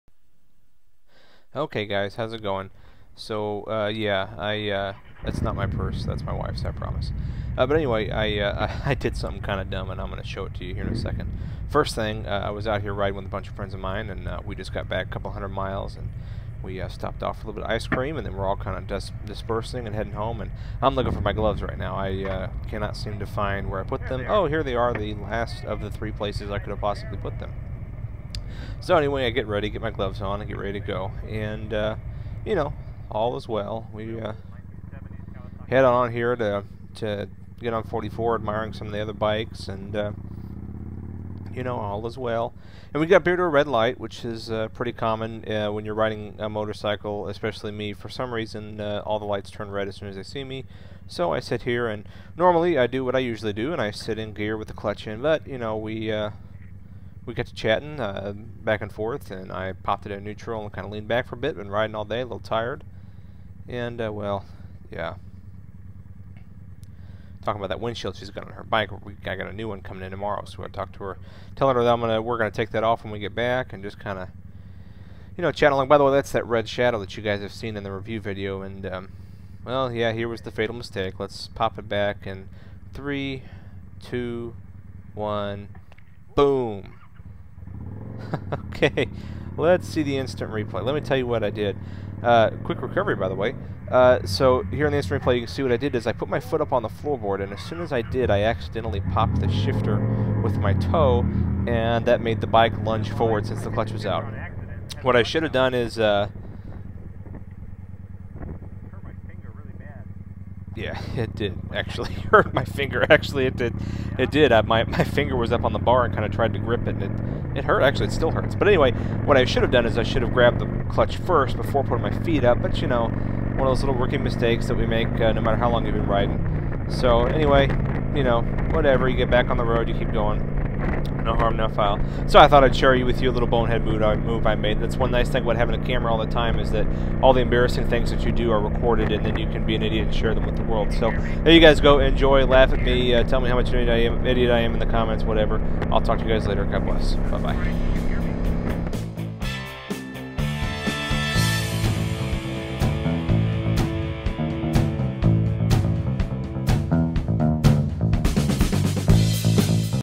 I Pulled a Bonehead Move
Whoops!

While riding home, I put my feet up on the floorboard and popped it into gear!  Eek!  No harm no foul; but figured y'all might appreciate seeing me make a fool of myself!

-John